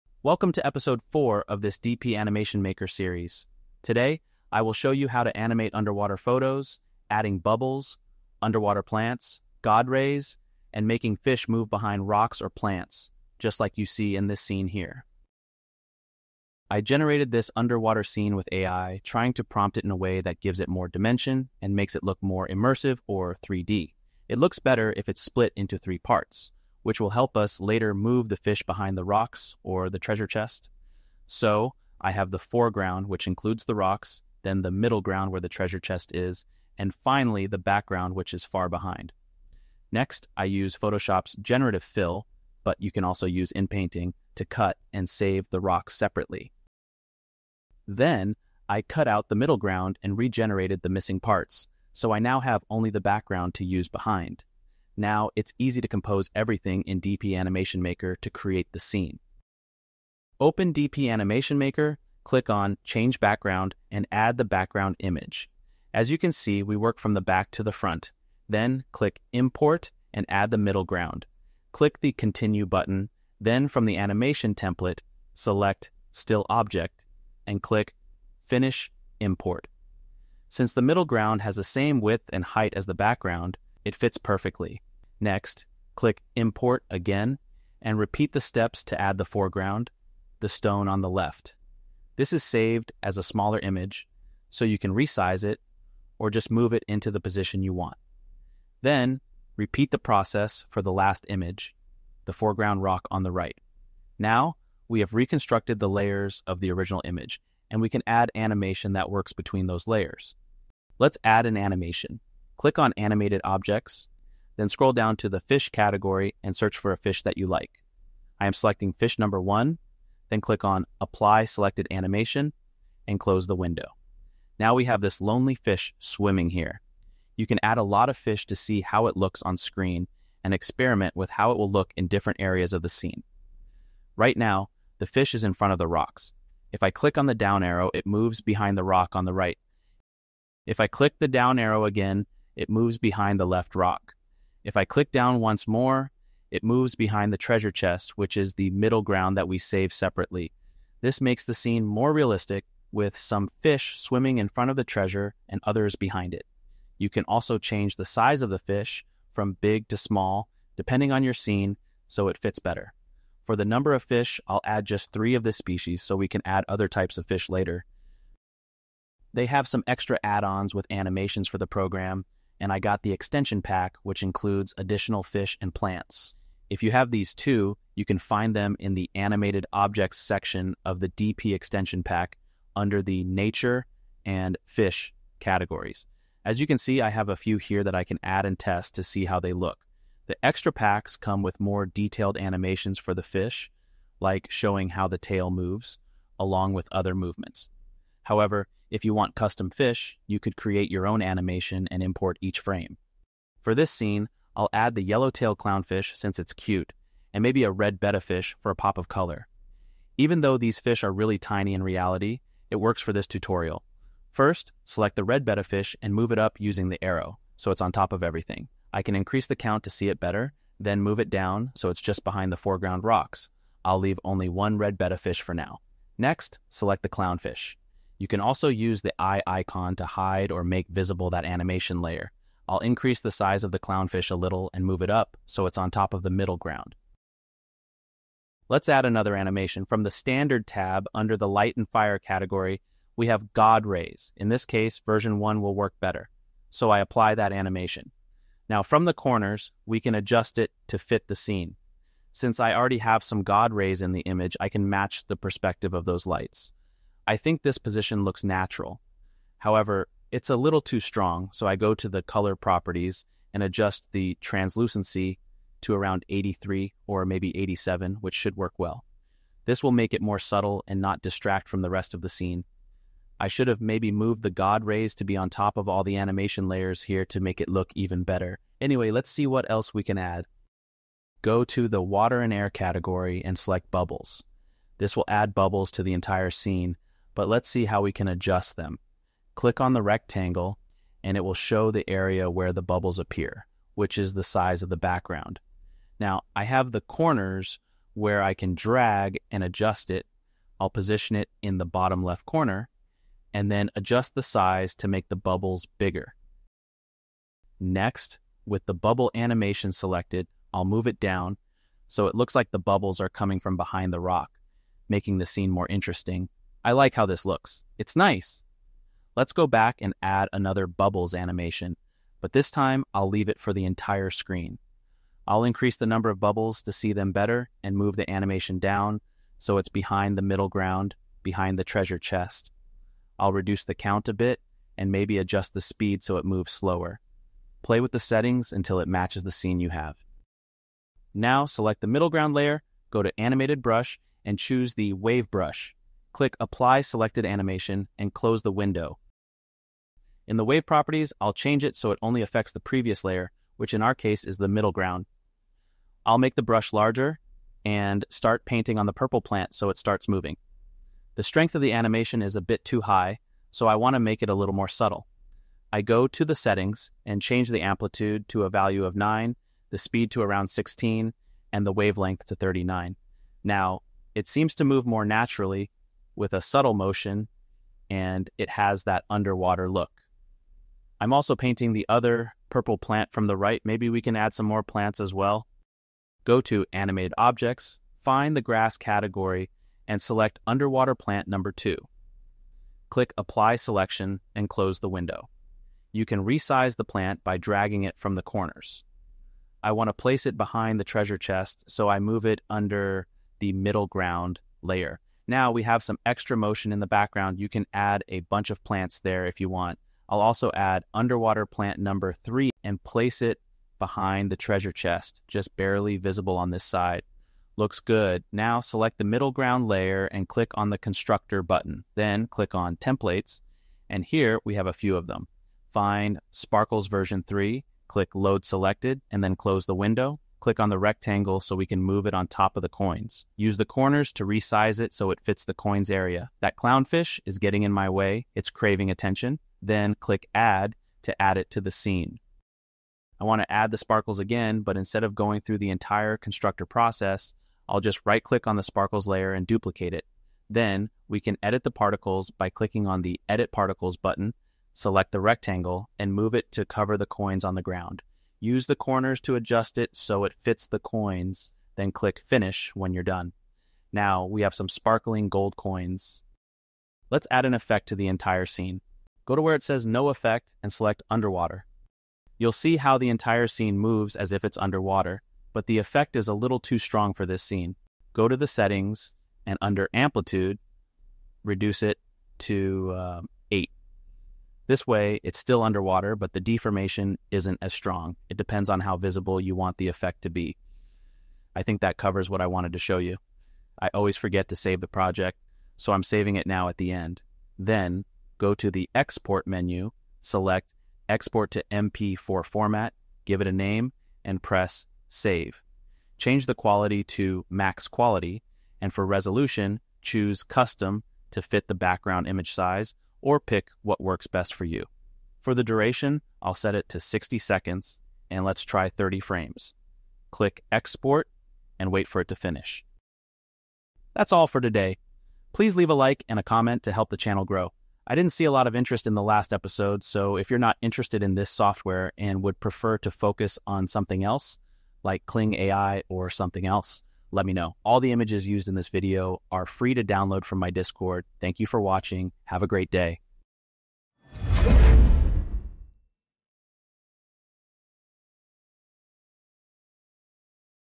DP Animation Maker Ep. 04: Underwater Photo Animations Made Easy!
In this episode of the DP Animation Maker series, learn how to bring underwater photos to life with immersive animations! Follow step-by-step as we:

- Split the image into foreground, middle ground, and background layers for realistic depth.
- Animate fish swimming behind rocks, treasure chests, and plants.
- Add dynamic god rays, moving underwater plants, bubbles, and sparkling effects to gold coins.
- Use advanced features like Animated Brushes, Extension Packs, and the "Underwater" scene effect.

Look for the ai2play-youtube channel and ai2play-resources to get the images and resources I used in this episode.